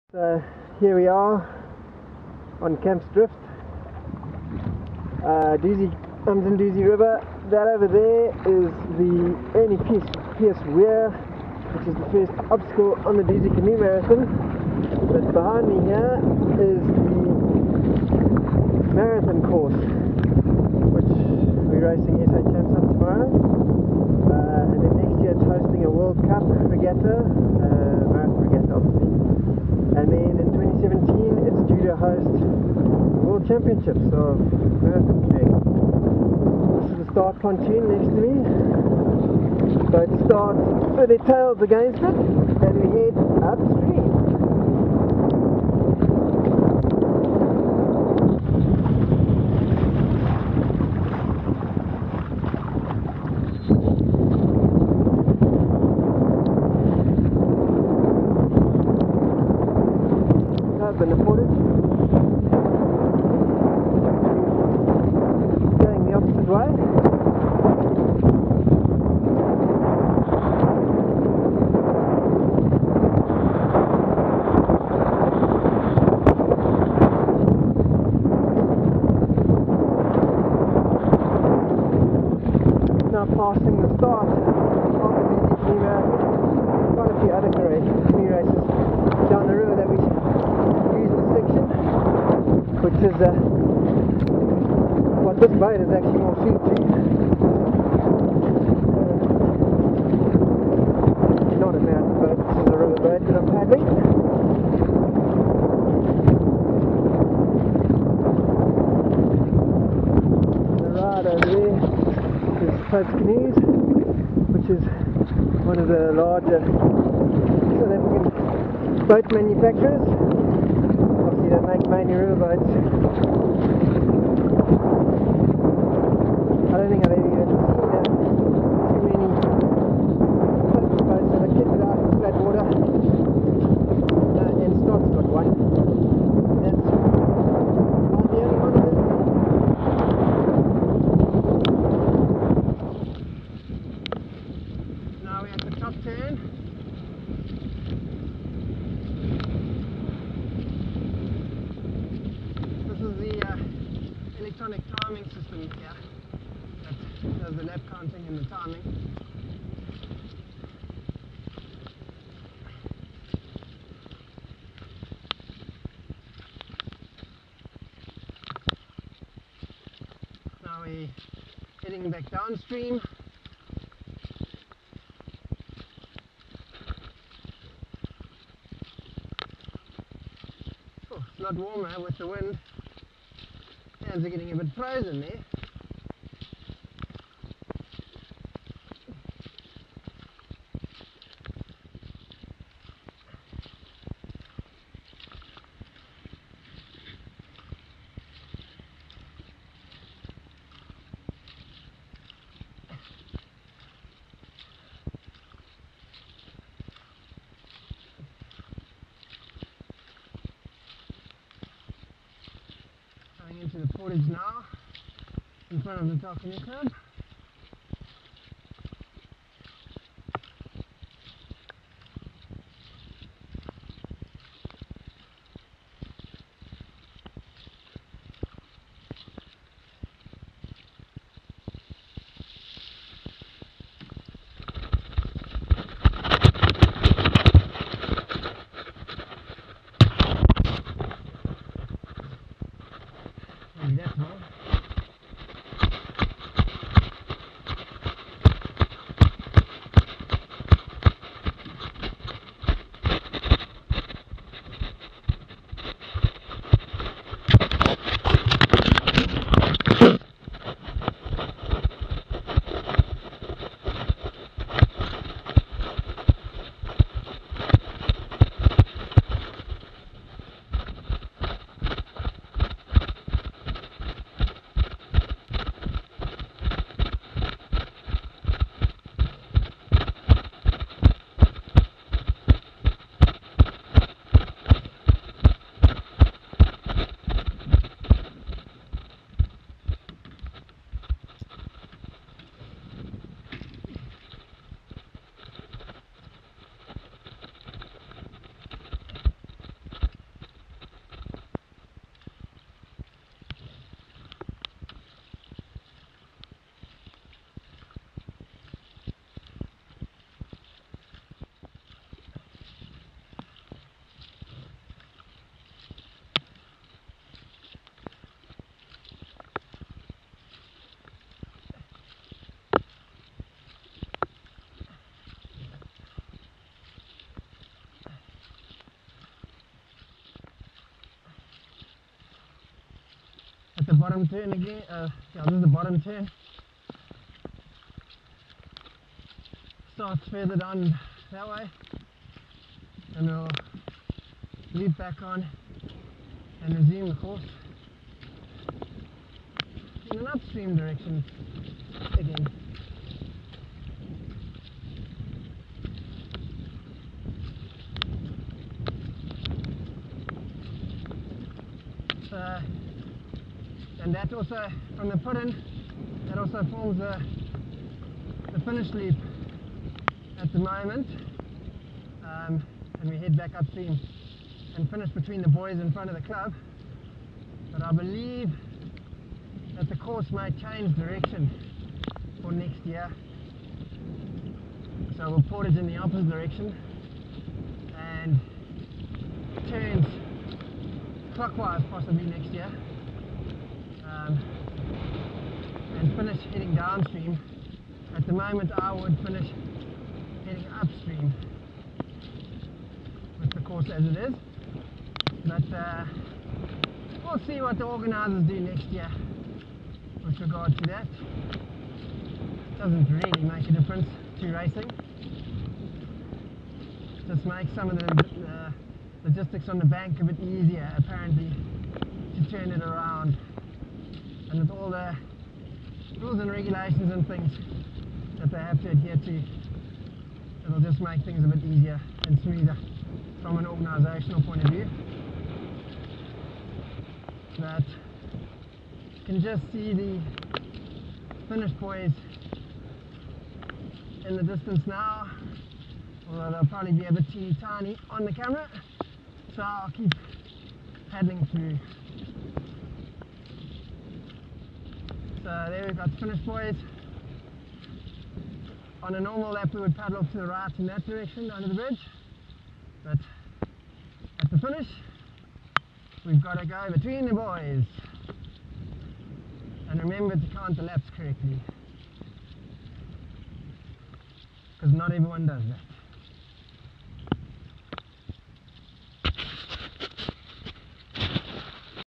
Camps drift marathon course 2015
A look at the marathon course due to host the 2017 ICF World Marathon Championships.
With GPS data included.
For more GPS data, log onto connect.garmin.com and search for "Camps Drift Marathon Course" under the SEGMENTS section.